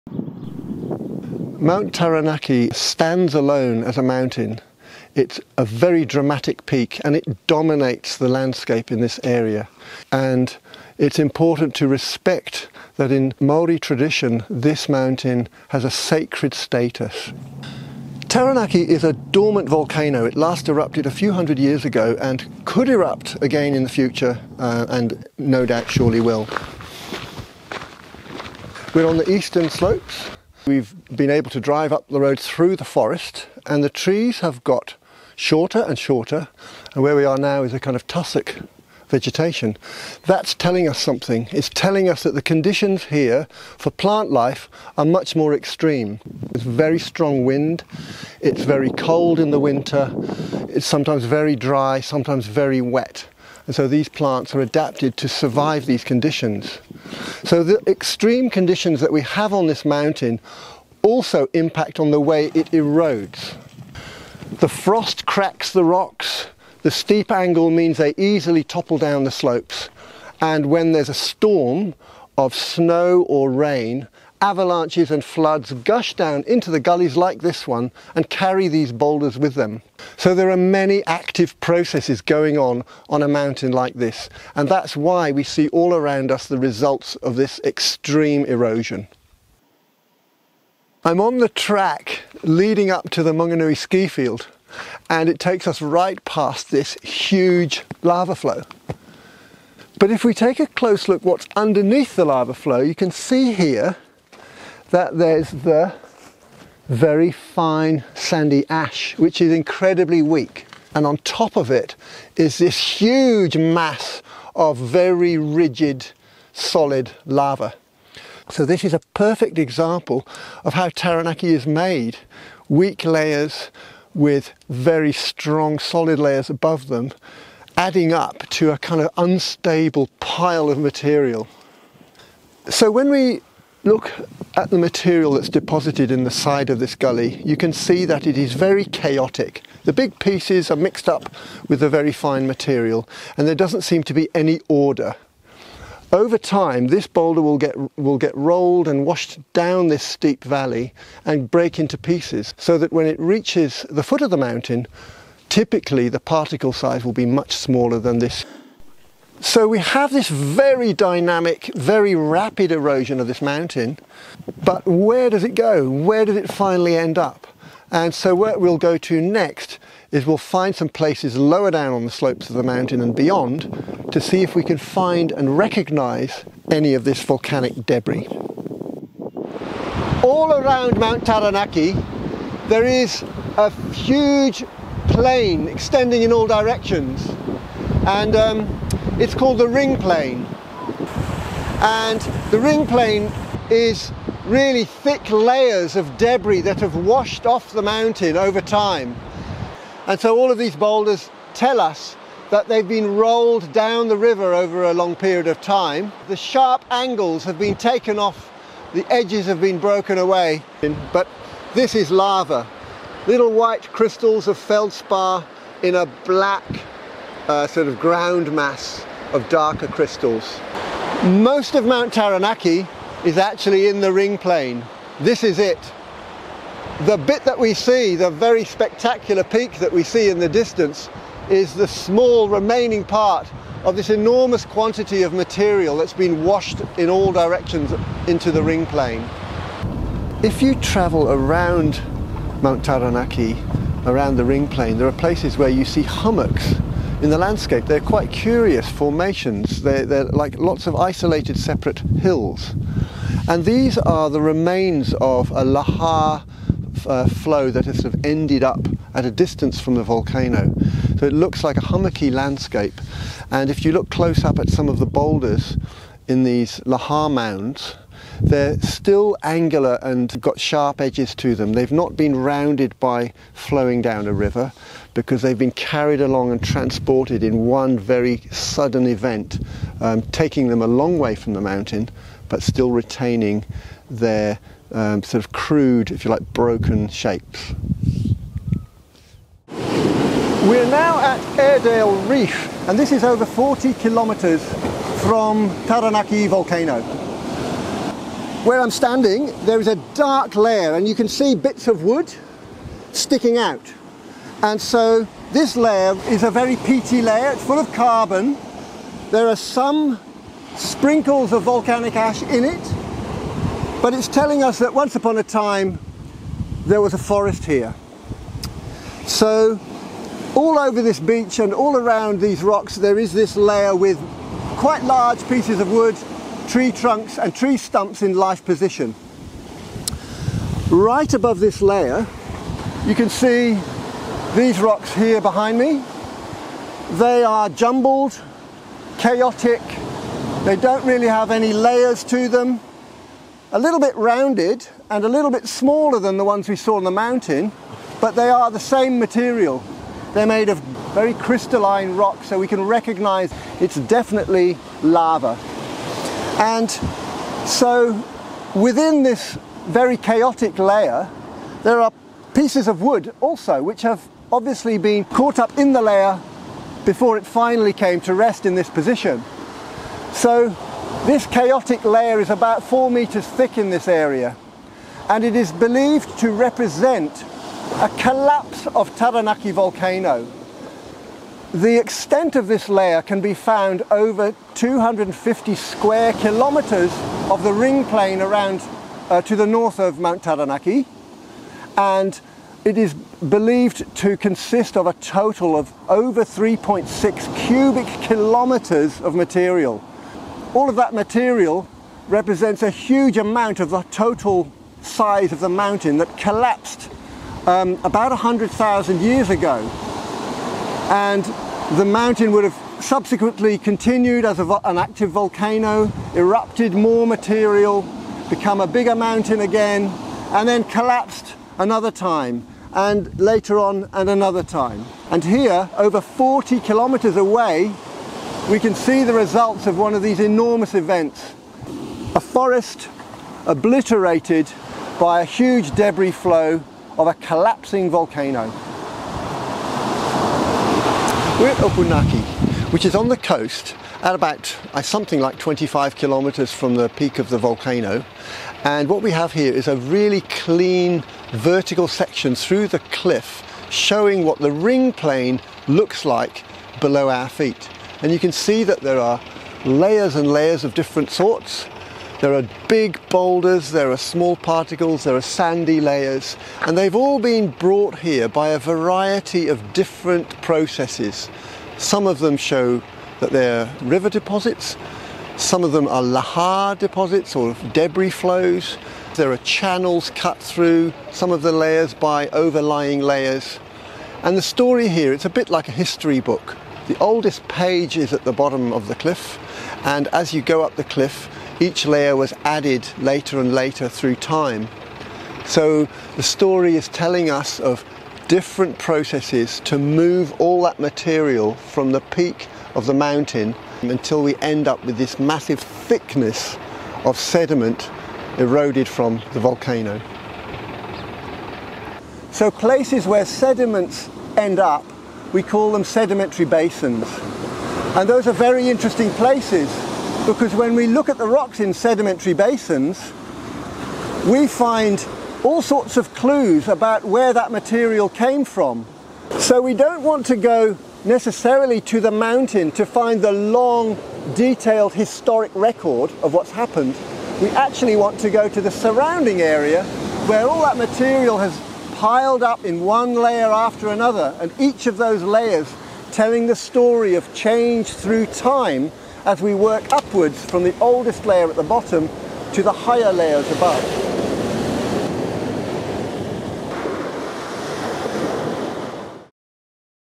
Erosion and Sedimentation of Taranaki Volcano
Taranaki Volcano stands in isolation above its own ring plain on the western side of New Zealand's North Island. It is a stratovolcano, made of layers of hard lava with soft ash sandwiched between them. This makes for a very unstable mountain and occasionally leads to massive collapses of the volcano.

Keen to massively BOOST YOUR SCIENCE COMMUNICATION IMPACT?

Visiting schools to talk science? This is how to TURN INFORMATION INTO INSPIRATION!

Video supported by GNS Science. We acknowledge the cultural significance of Maunga Taranaki to all iwi of Taranaki and thank tangata whenua (indigenous peoples) for the privilege of engaging with the ancestral landscape and telling the geological story. Thanks also to Making Movies Co, Auckland for aerial imagery and the assistance of the staff of the Ngāmotu/ New Plymouth Office of the Department of Conservation Te Papa Atawhai New Zealand.